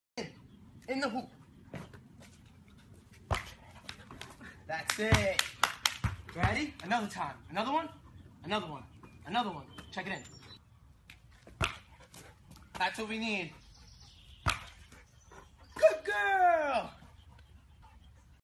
Amazing Shepherd Dog Playing BasketBall
Watch this skilled dogs playing basketball like an NBa star